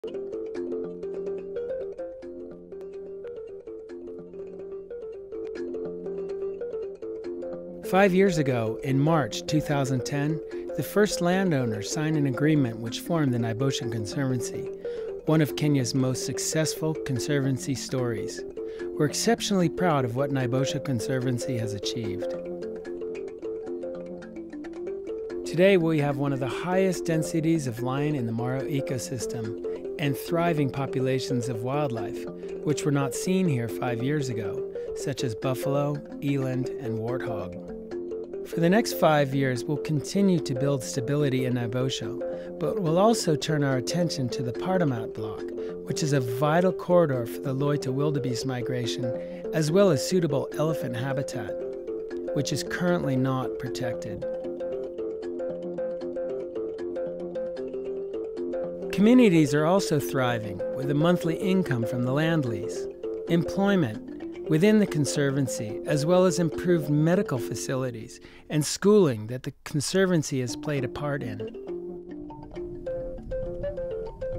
Mara Naboisho Conservancy: celebrating 5 years of conservation
In this short film we celebrate everything the Mara Naboisho Conservancy has achieved during the past 5 years.